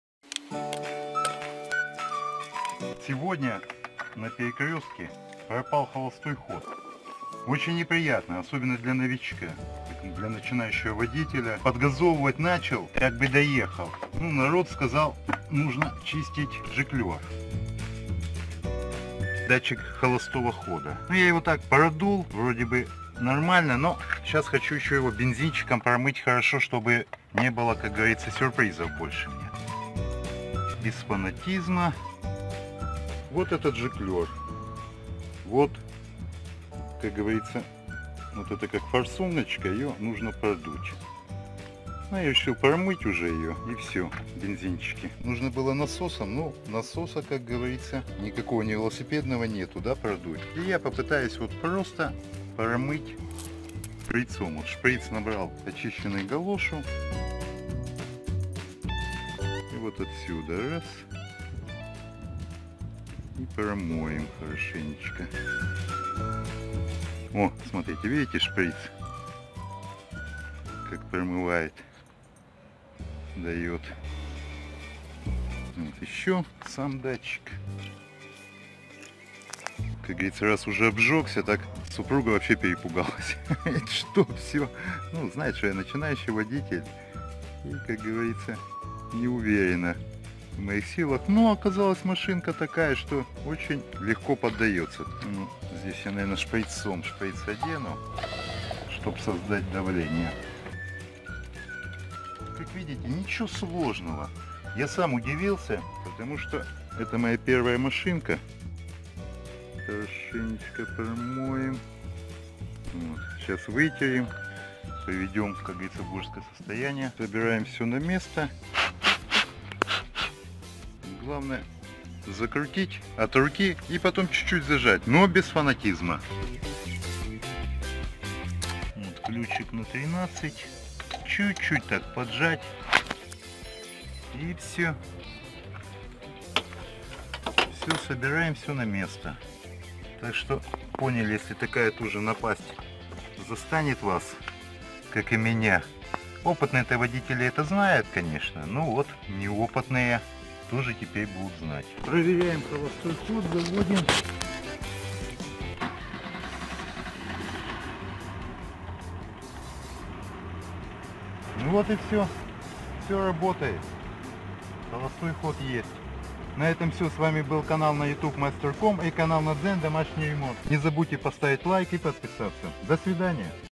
Если двигатель стал глохнуть на холостом ходу. Простое решение
Простой способ прочистить жиклёр электромагнитного клапана карбюратора, если нет насоса.